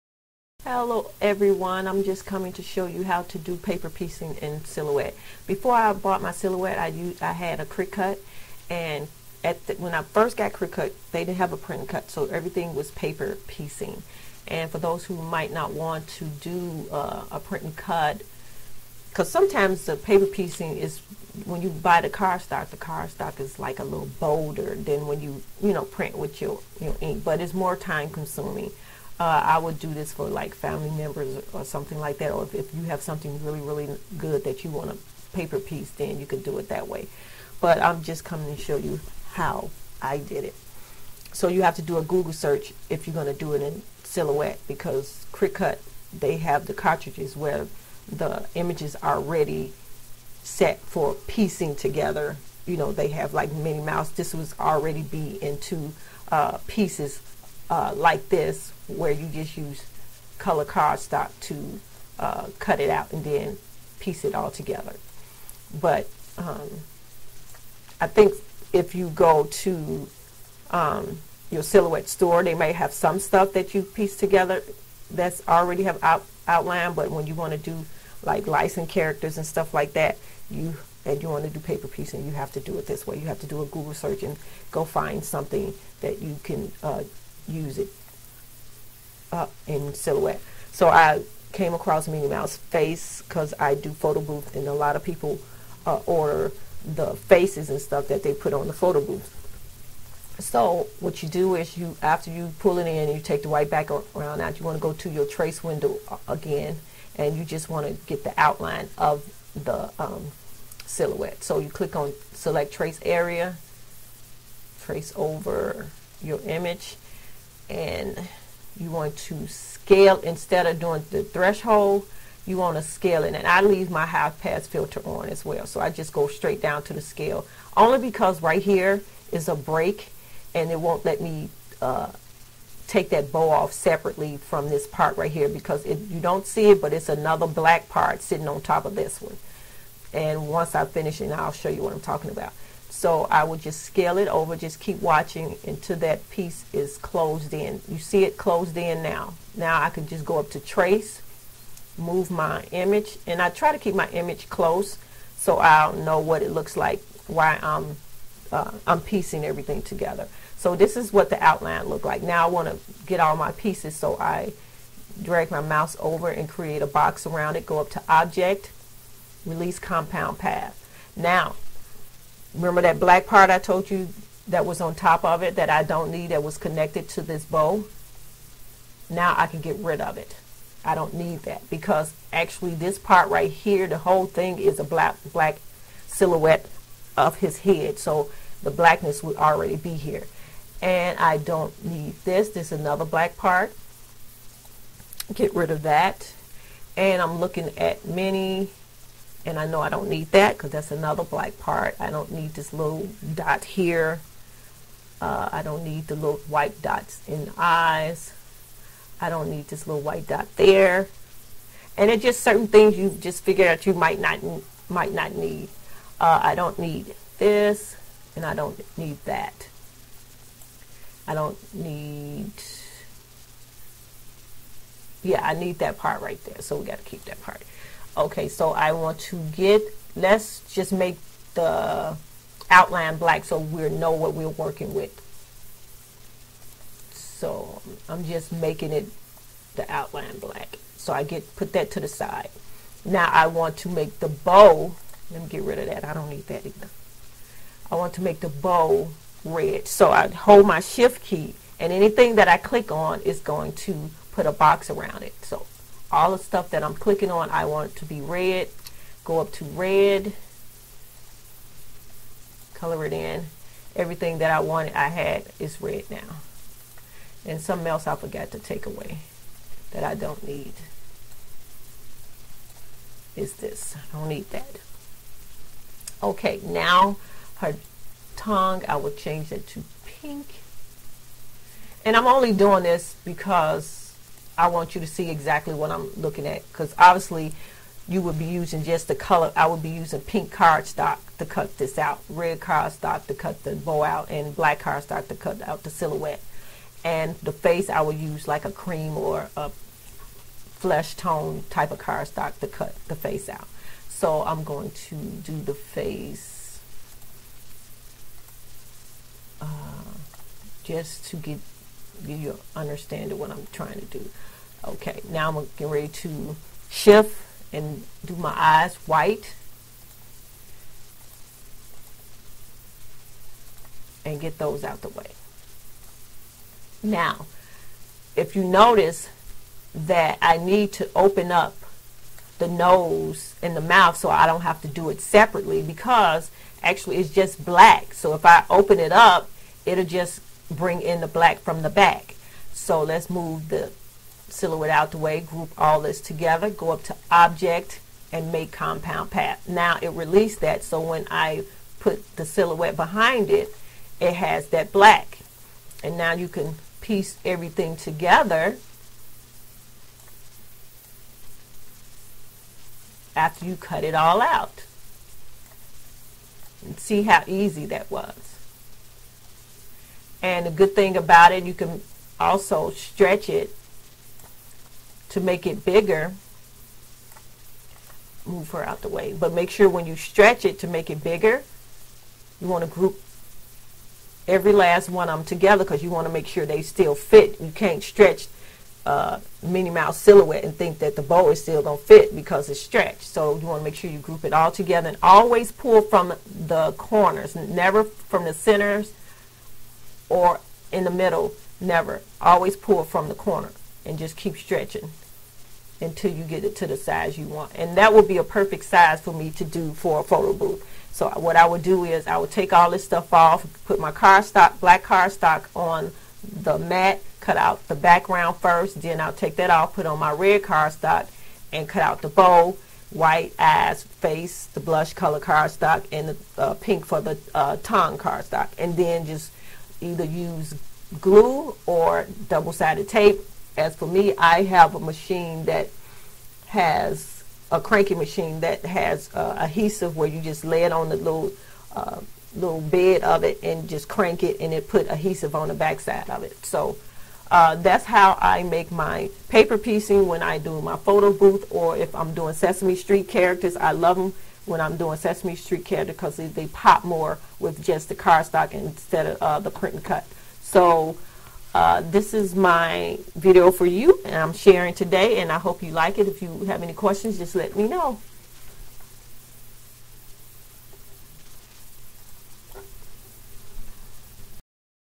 Paper Piecing with Silhouette Cameo
How to paper piece google image using your Silhouette Cameo